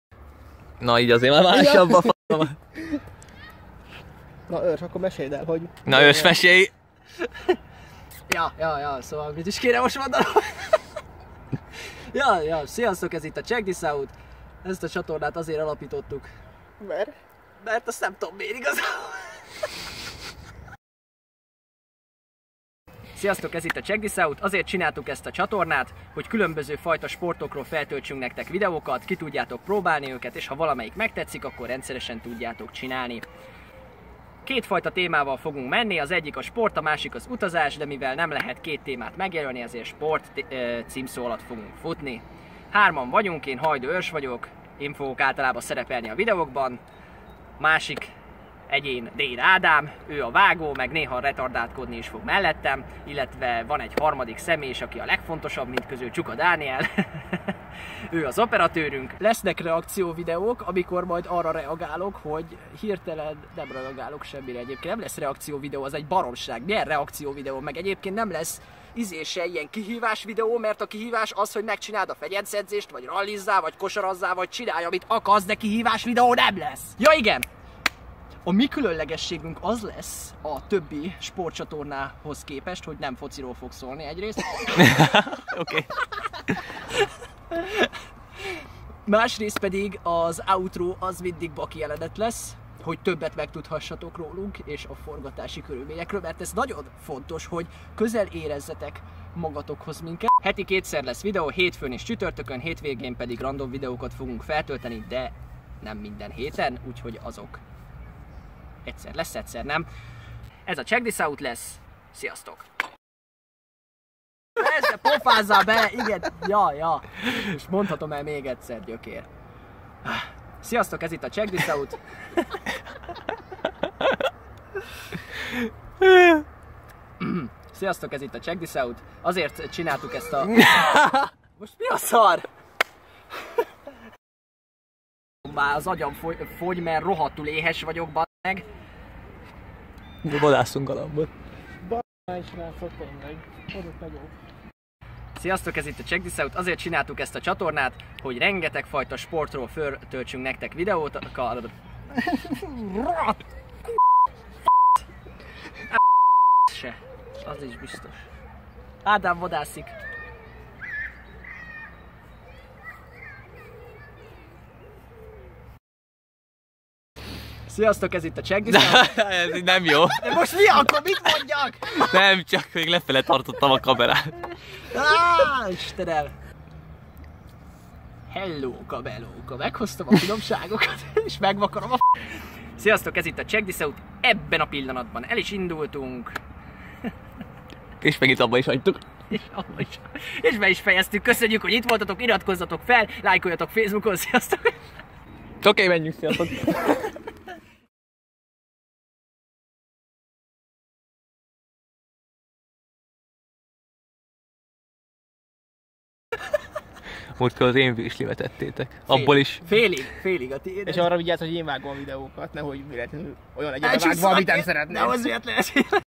Mi a tököm az a CheckThisOut? (felirattal) / CheckThisOut
Már megint egy újabb hülye csatorna, ami nézettségre áhítozik! Hát ezek vagyunk mi, a CheckThisOut!

Bemutatjuk magunkat, ahol elmondjuk, hogy miről szól ez a csatorna.